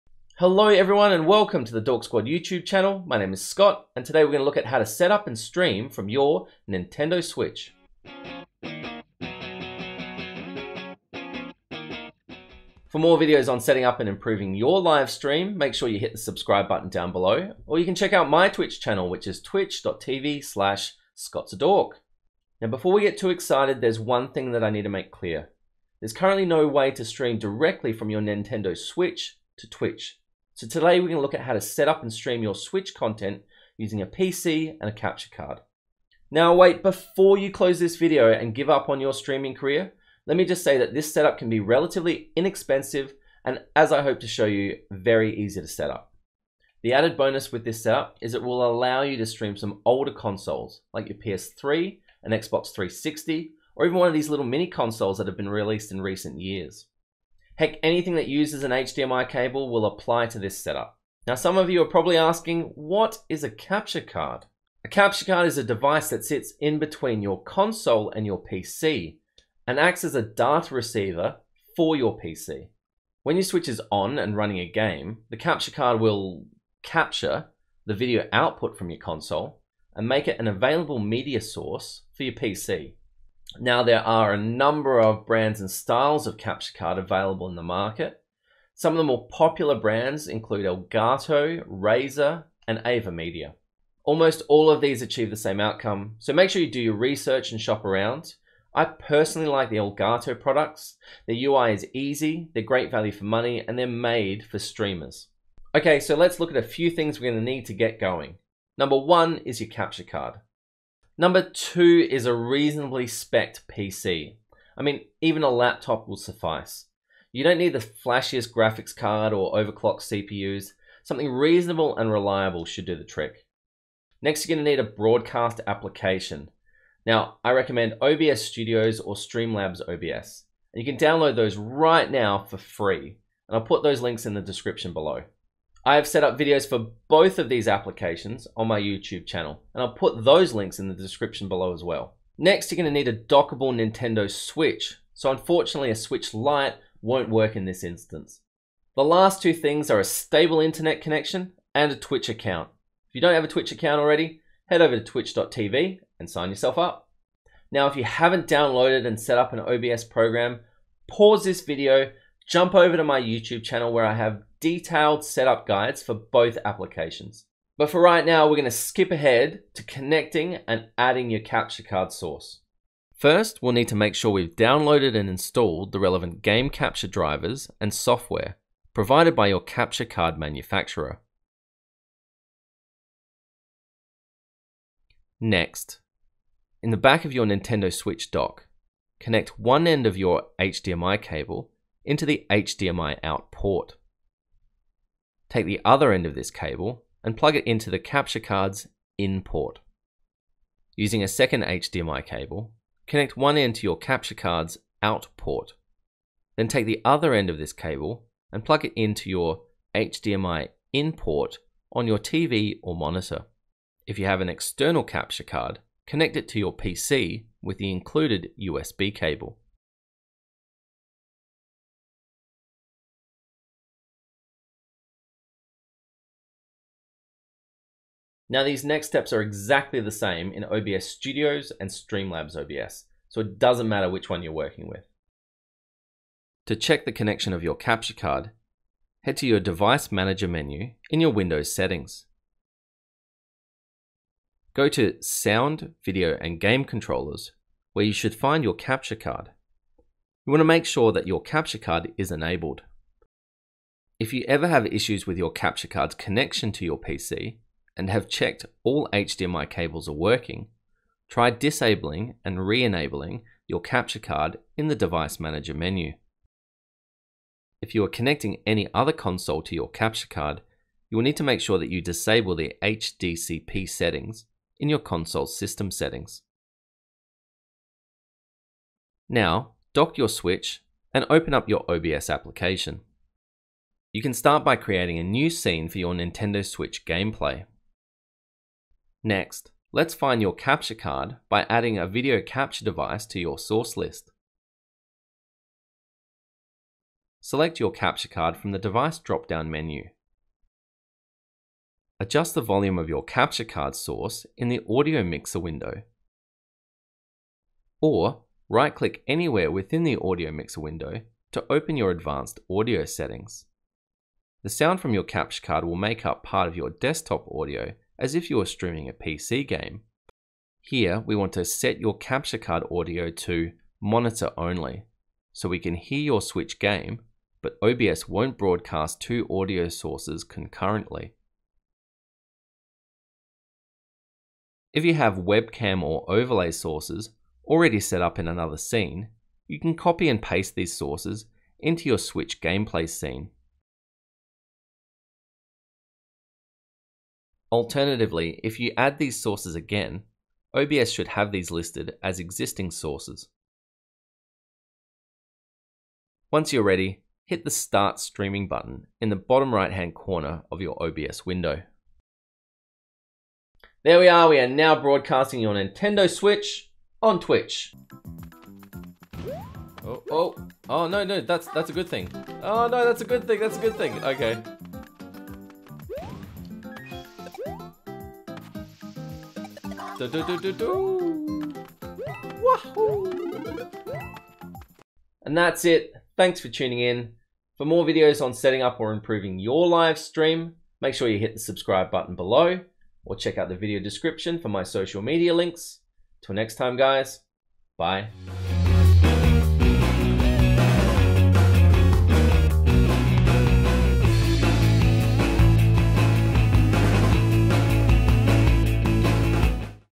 Stream to TWITCH on Nintendo Switch - Capture Card Setup Guide
How to Stream to TWITCH on Nintendo Switch - A Capture Card Setup Guide - This tutorial will guide you through how to live stream you Nintendo Switch content on Twitch, using a PC and a Capture Card. ⬇️TIMESTAMPS & LINKS⬇️

TIMESTAMPS
⏩ 0. Introduction 0:00
⏩ 1. What is a Capture Card? 1:01
⏩ 2. Getting Started 1:46
⏩ 3. Connecting Your Console 2:48
⏩ 4. OBS Settings 3:48
⏩ 5. Audio Settings 5:00
⏩ 6. Webcam Overlays 5:35
⏩ 7. Going Live 5:56